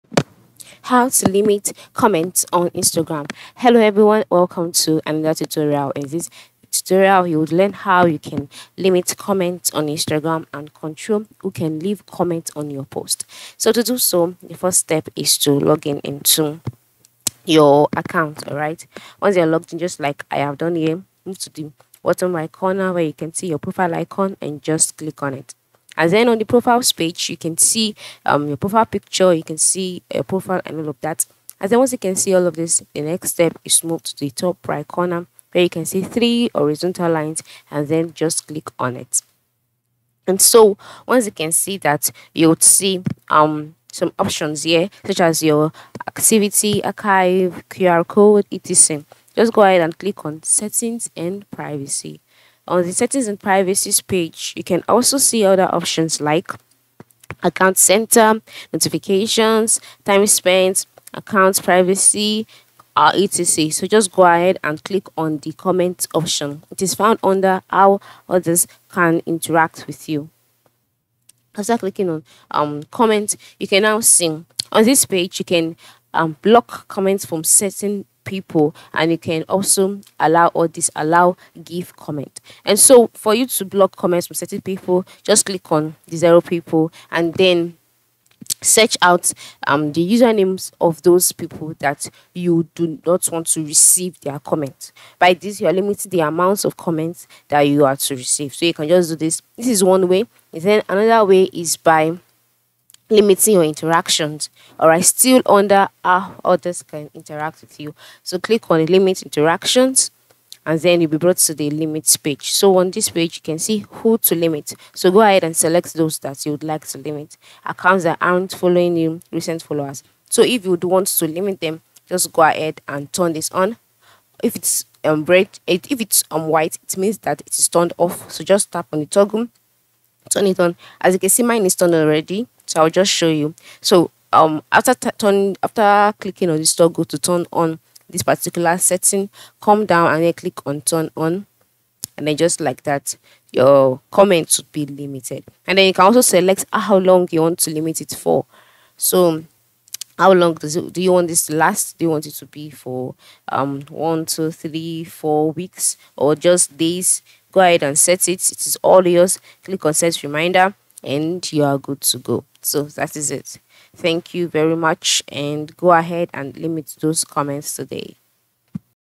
How to Limit Comments on Instagram (Easiest way)
In this video I will show you How to Limit Comments on Instagram. It would be good if you watch the video until the end so that you don't miss important steps.